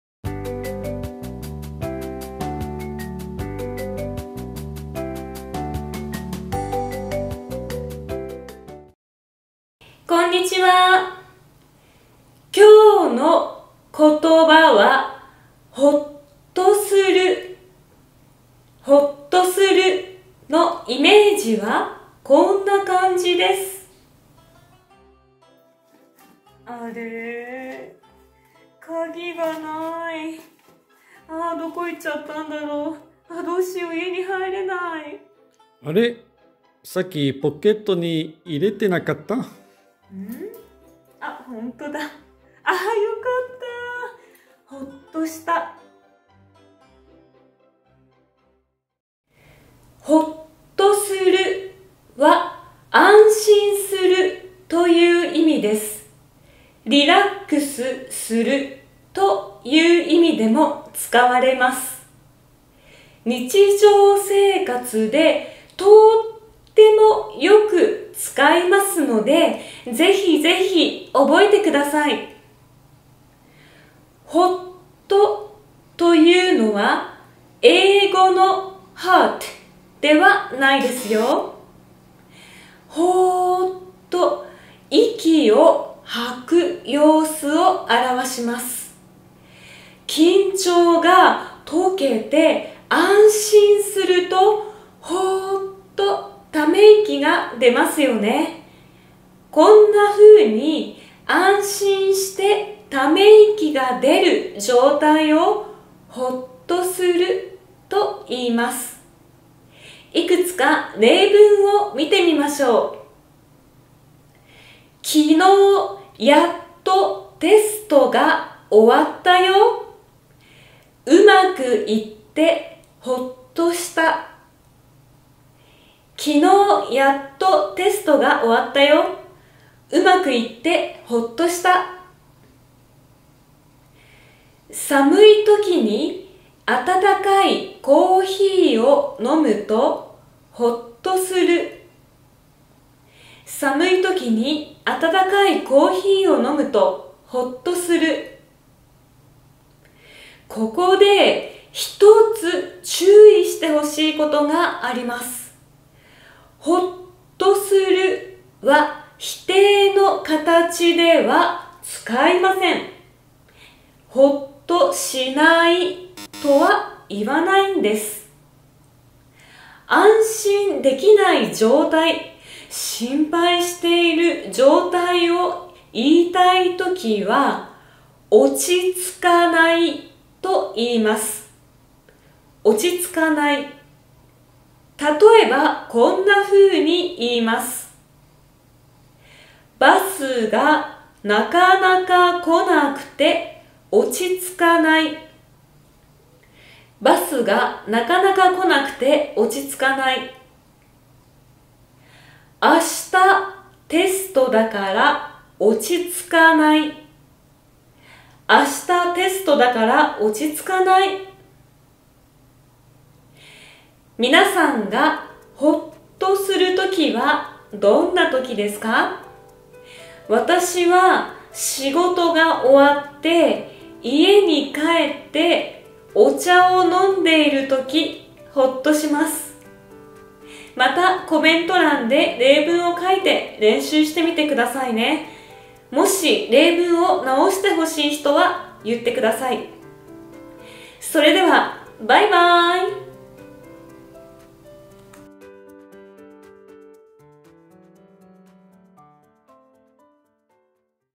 今日のことば vol.10 / The word of the day : Learning Japanese
Today's word is "hotto suru".
今日のことば「ほっとする」

Subtitles: Japanese with Furigana, English
字幕（じまく）：日本語（ふりがな）、英語

Videos to recommend / おすすめのビデオ

Script of this video is available in my blog.
動画のスクリプトはブログにあります。